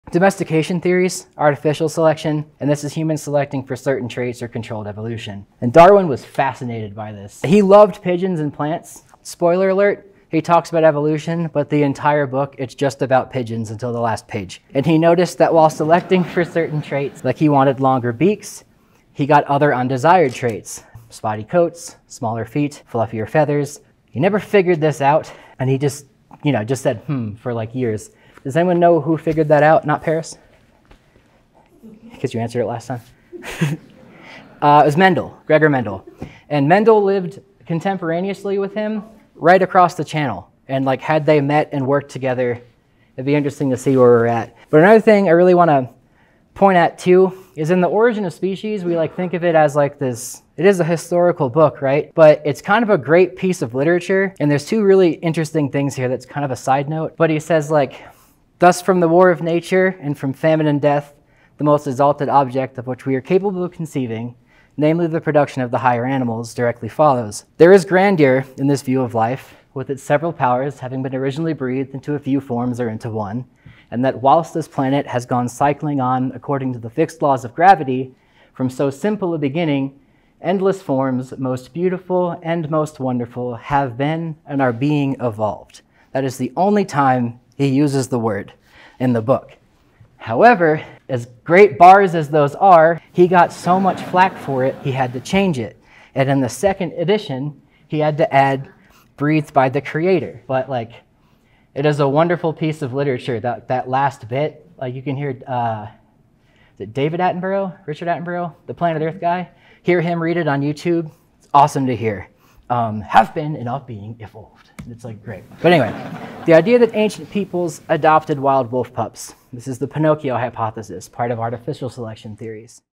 Charles Darwin‘s most famous yet most controversial quote ￼
Charles Darwin

And graduate school, at least for anthropology, every student has to take a biological theory class. And in that class, you learn quite a bit about the history of biological thought. From the ancient Greeks, to the Renaissance, to the Darwinian revolution(?), and tell the science of biology and biological thought has evolved over the years itself.

I always talk about Darwin in my lecture because he was actually fascinated by dogs and wrote quite a bit about them in his books. A lot of his postulations turned out to be incorrect, but he was thinking along the right lines.

Here is one of my favorite quotes in the English language, that happens to be the final quote of On the Origin of Species.